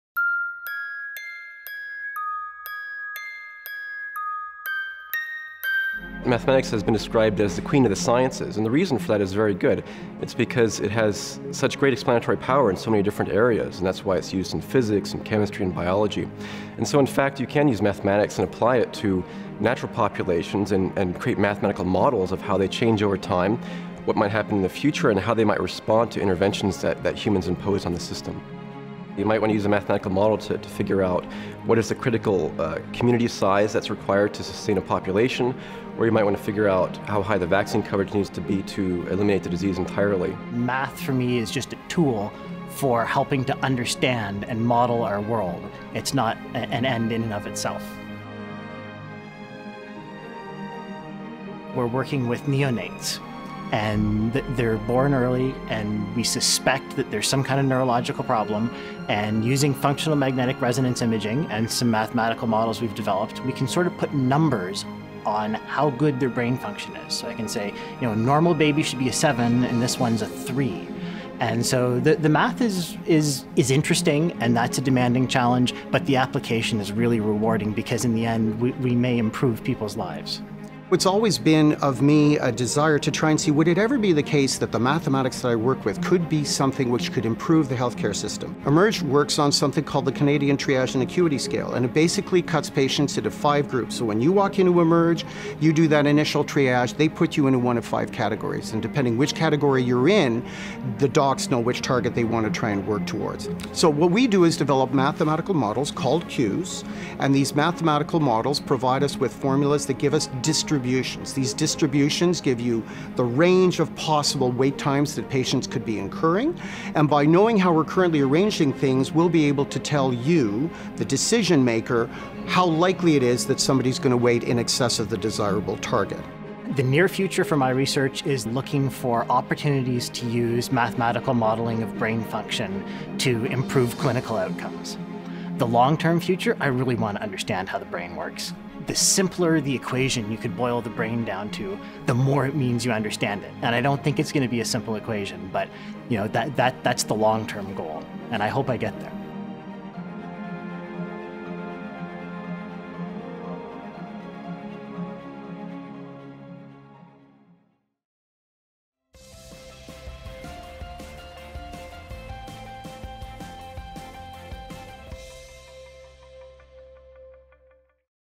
Yes, You Do Use Math in Everyday Life
Despite your schoolyard skepticism, you are using math as an adult. Researchers are using it too, to confront some of society's big issues.